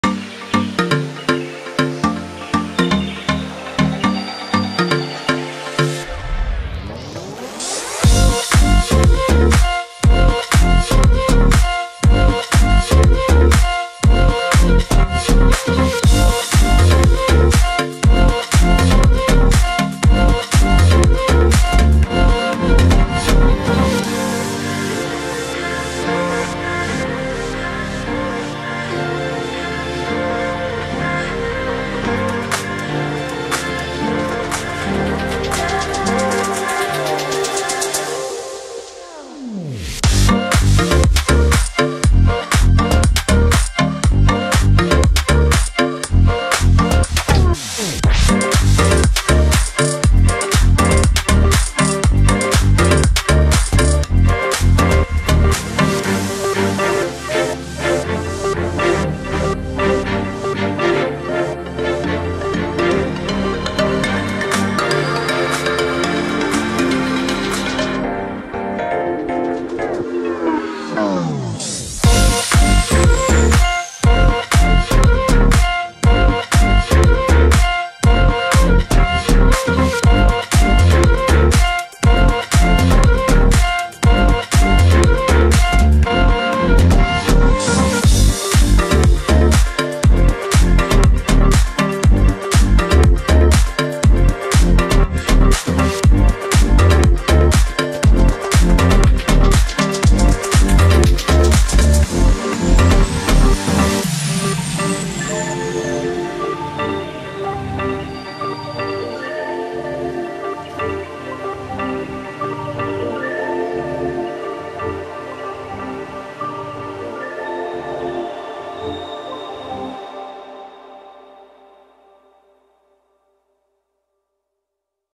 How to protect your car key suggestions Peugeot 3008 2021-2024 ACTIVE PACK
Your Peugeot 3008 facelift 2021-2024 key needs to be protected from scratches and not to where off. You need to cover it with a glove so not that happens. I prefer leather style but you can use silicone toot